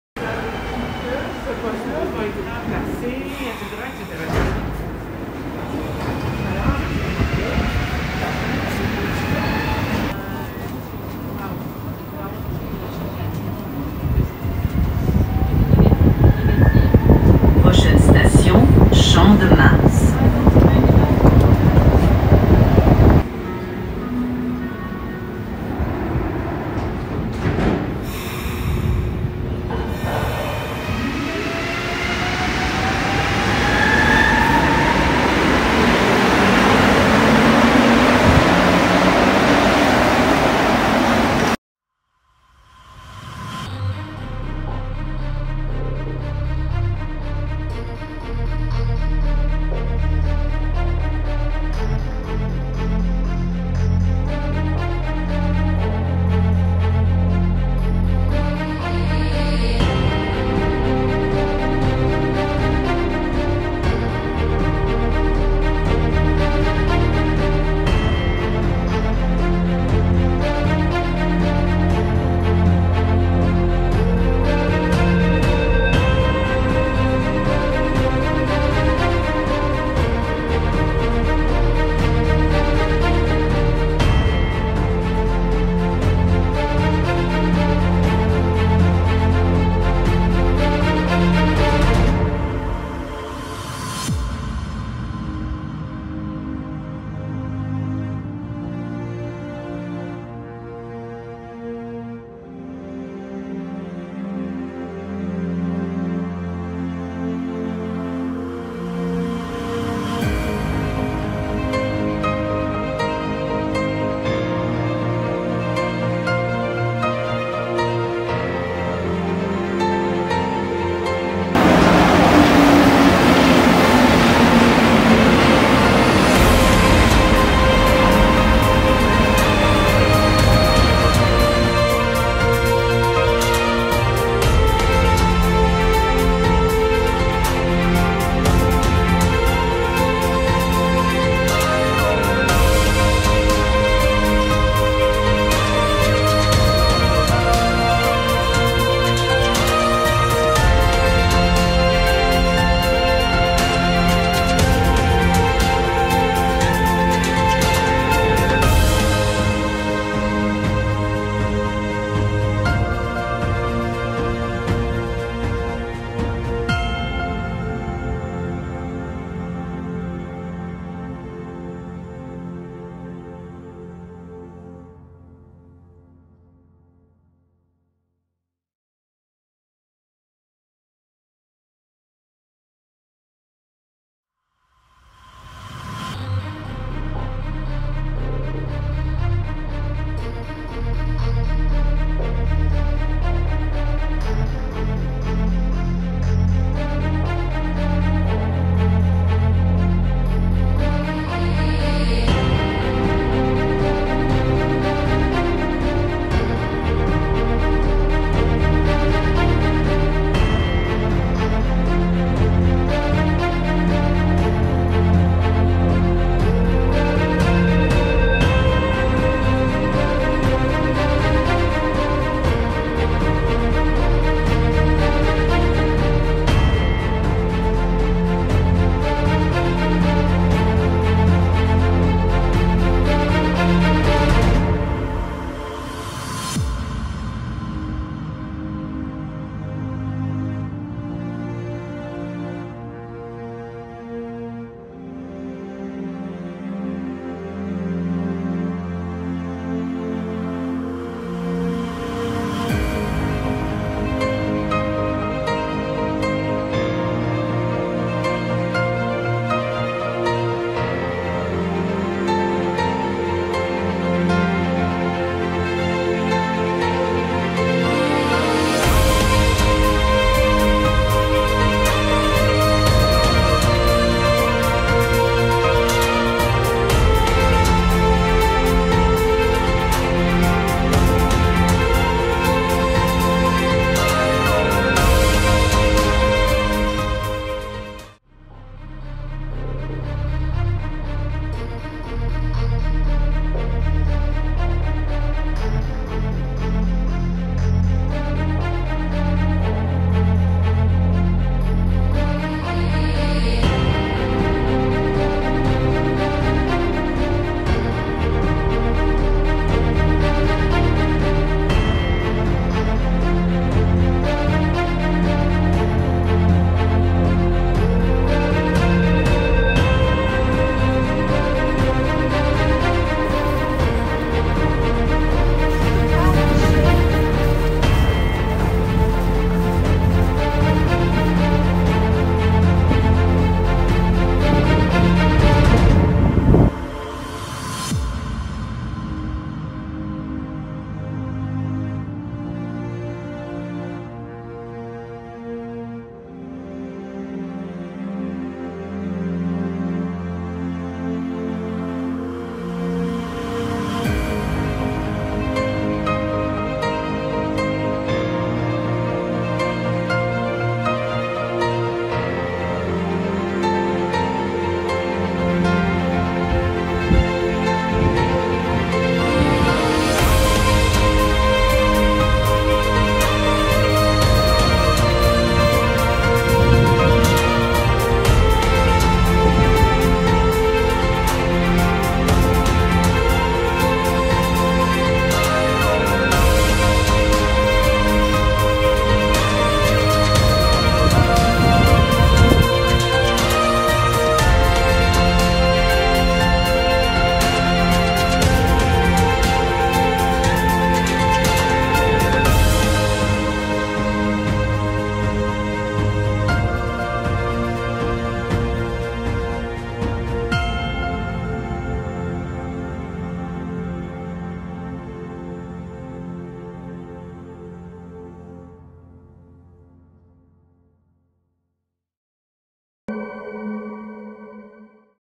Montréal Work-of-Art Metro
Champ-de-Mars

The simple interior design is backdrop for Marcelle Ferron's stunning stained-glass window. This glorious work lets brilliantly coloured light splash into the mezzanine and platforms from three sides of the kiosk. One of the most beautiful works of art in the metro.